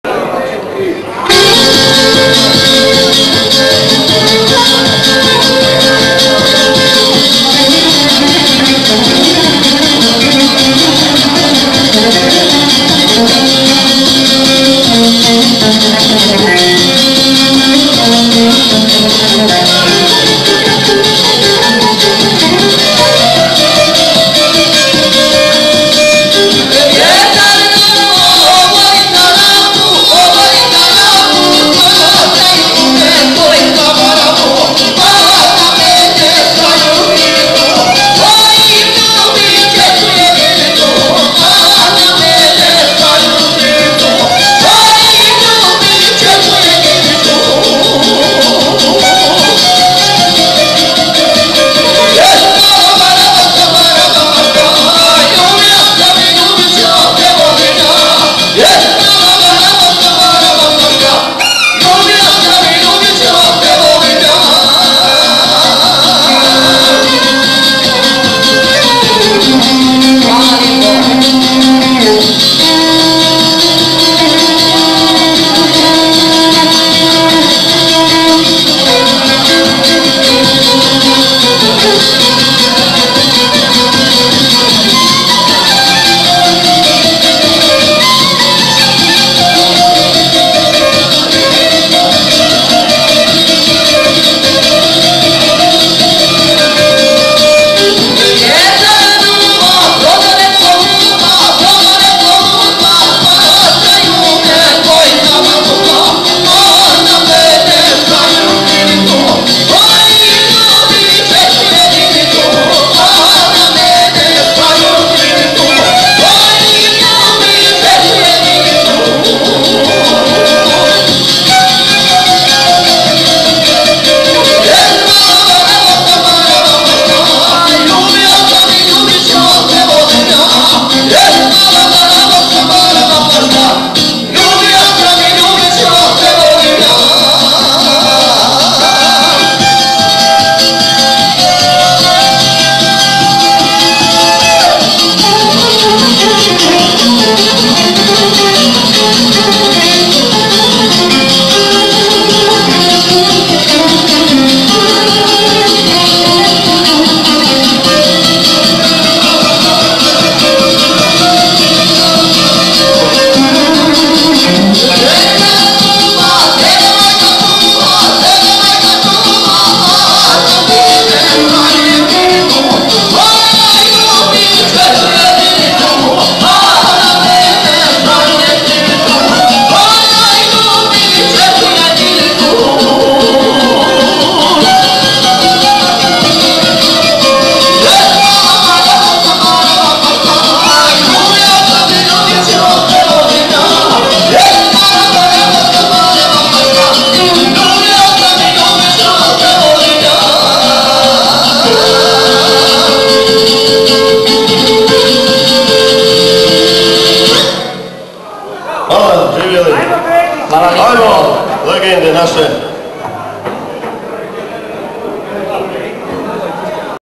26 Jovo iJagoda na koncertu BRAĆE BEGIĆ
Jedna od muzičkih nimera koju su pjevali Jagoda i Jovo na koncertu velikih derventskih izvornjaka i neponovljivih legendi Braće Begića.